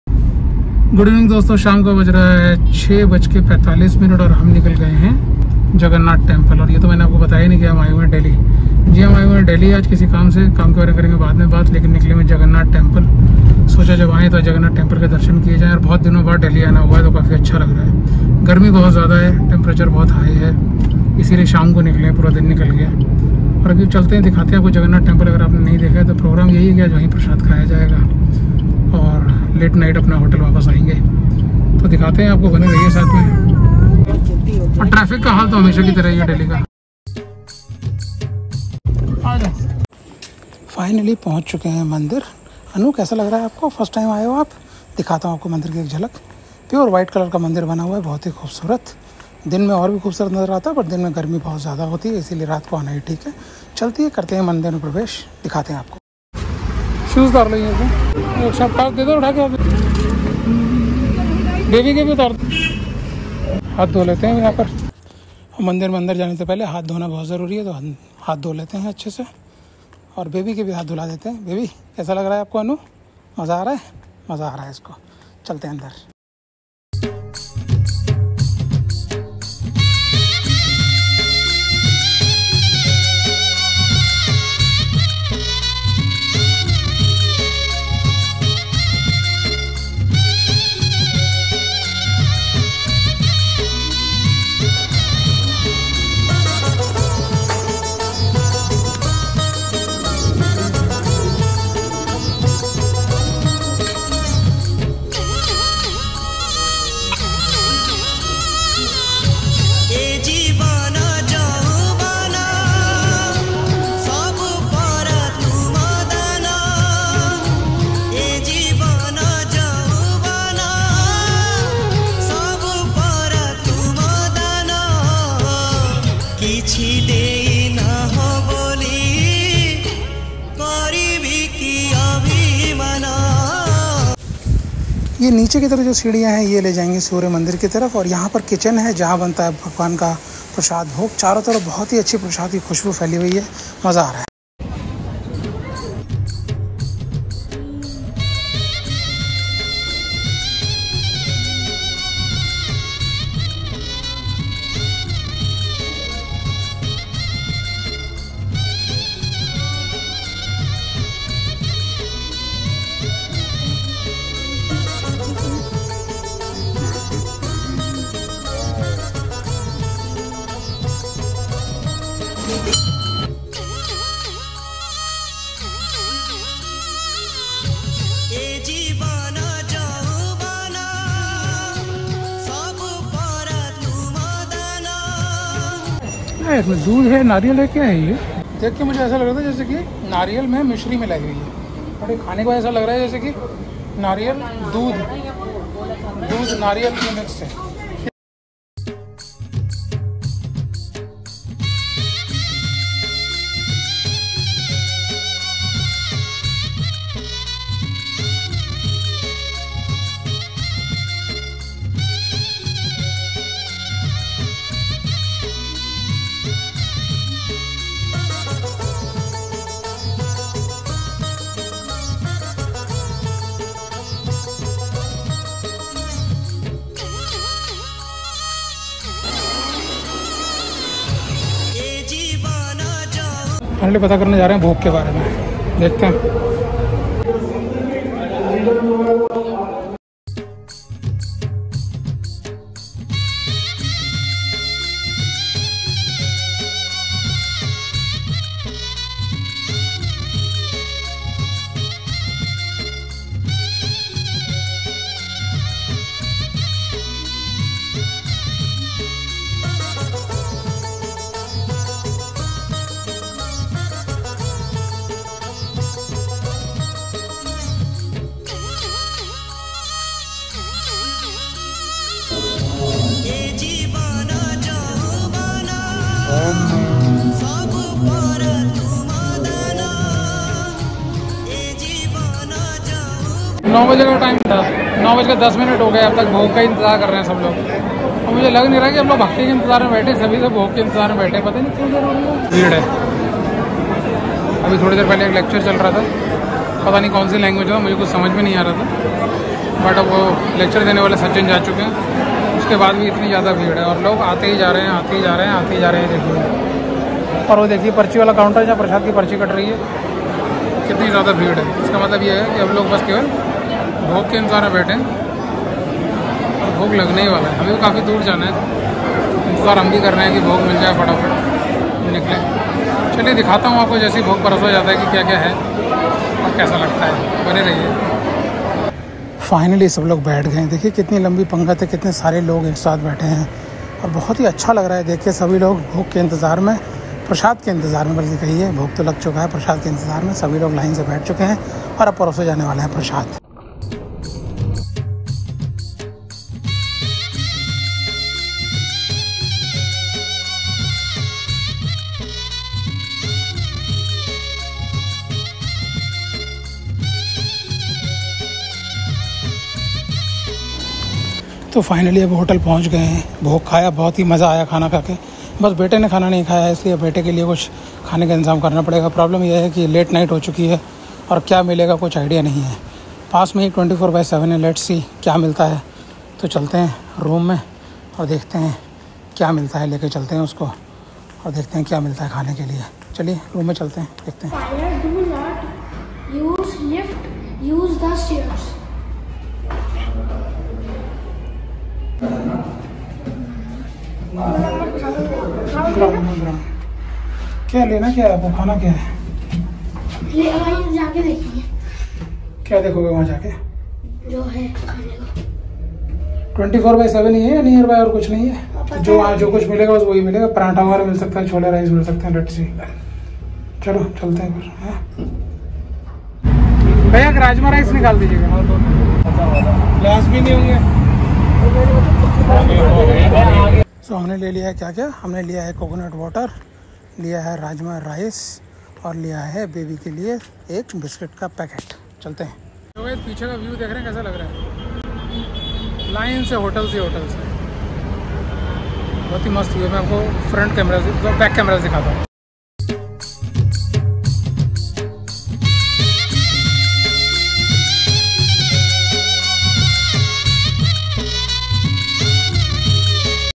Prasad Kha Ker Maza Aa Gya 🥰
Shri Jagannath Mandir Delhi mein prasad kha ker maza aa gaya